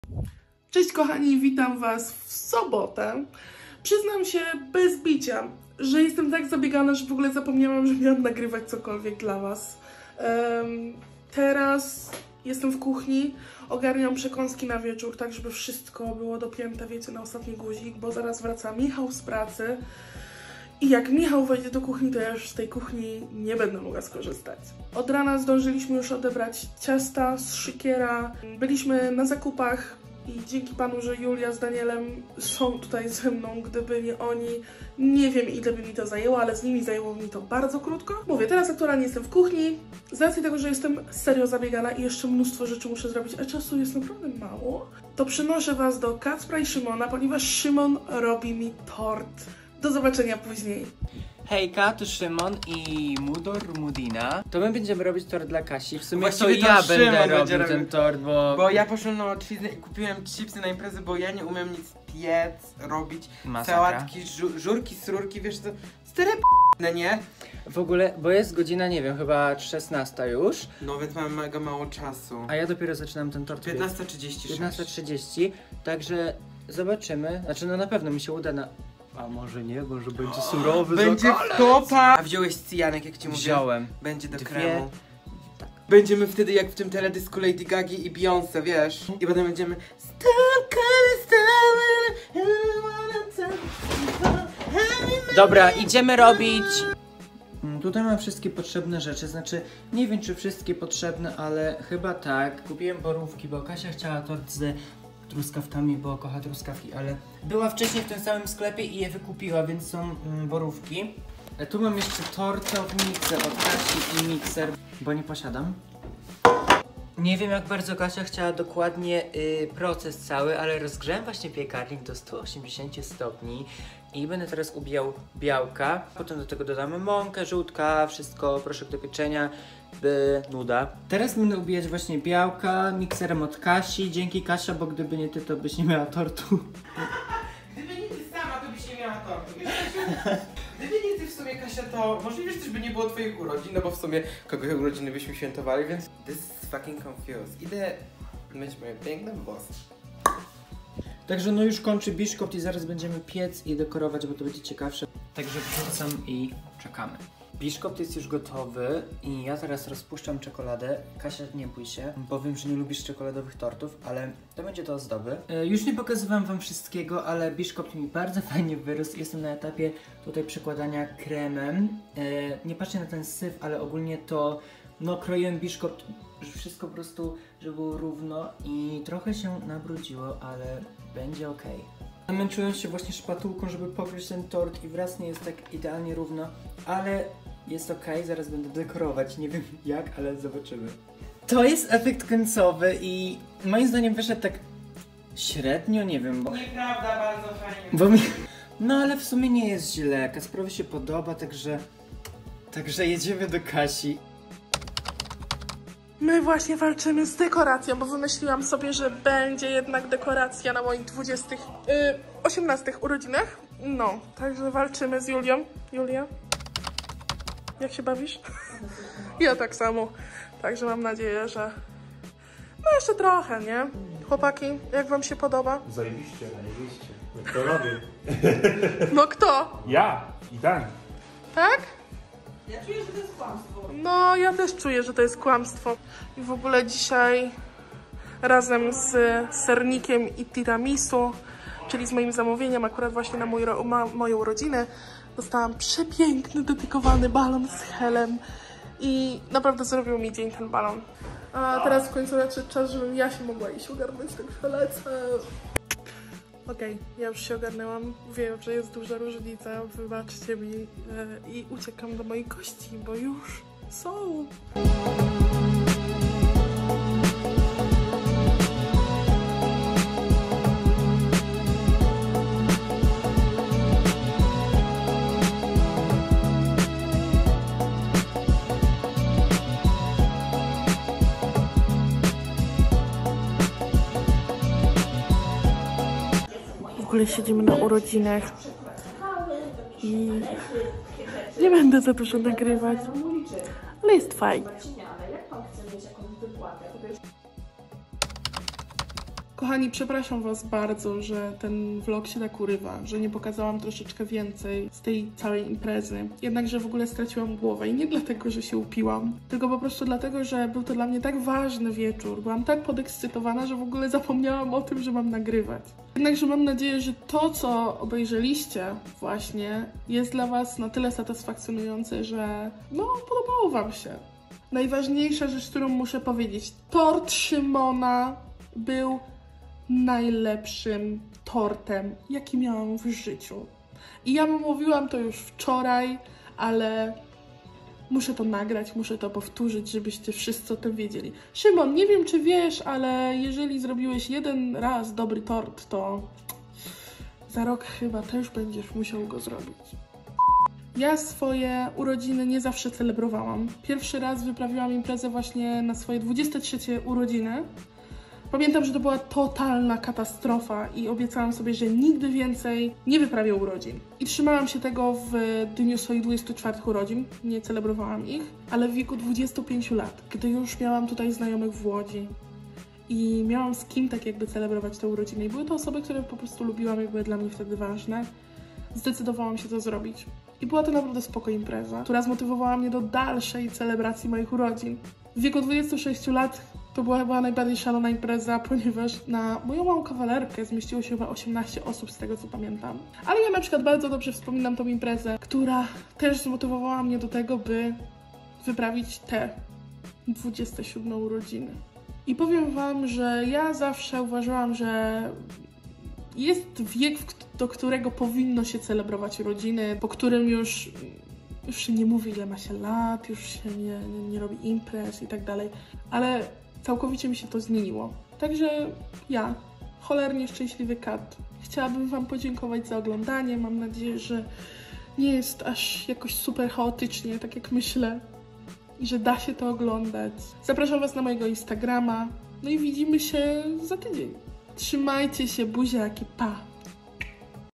Moja mini impreza urodzinowa!
Cześć, jestem FATKAT i jestem tatuatorką.
Tym razem vlog bez zabarwienia tatuatorskiego 🤭
Mam nadzieję, że mi wybaczycie!

Dajcie znać jak Wam się podoba i do zobaczenia w następnym filmie!